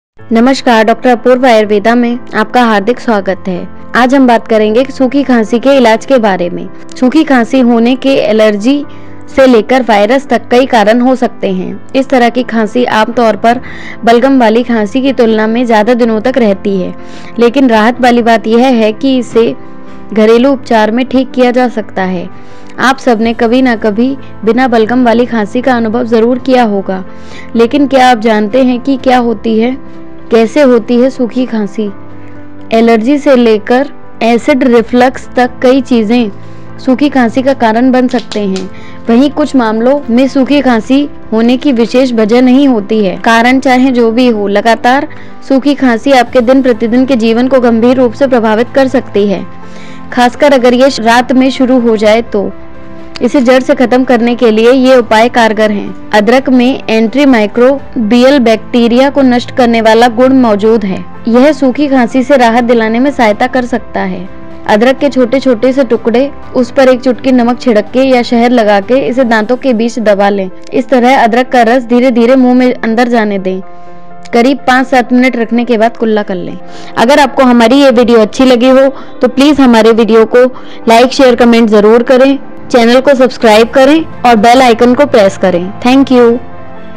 लगातार चल रही खॉंसी चाहे कैसे भी हो सूखी या कफ वाली, एक मिनट में तुरंत बंद | Home Remedy for Cough
सूखी खांसी बेहद खतरनाक होती है। खांसते-खांसते पूरे पेट में और पसलियों में दर्द होने लगता है। खांसी वैसे तो जुकाम और फ्लू के साइड इफेक्ट से होती है, लेकिन कई लोग मौसम बदलने की वजह से ही इसकी गिरफ्त में आ जाते हैं। अगर आप लंबे समय से सूखी खांसी से परेशान हैं, तो यहां कुछ घरेलू नुस्खे बताए जा रहे हैं जो झट से खांसी को खत्म कर देंगे।

Welcome to Dr. Apurva Ayurveda.

lots of love ever and always ❤️

Dr. Apurva Ayurveda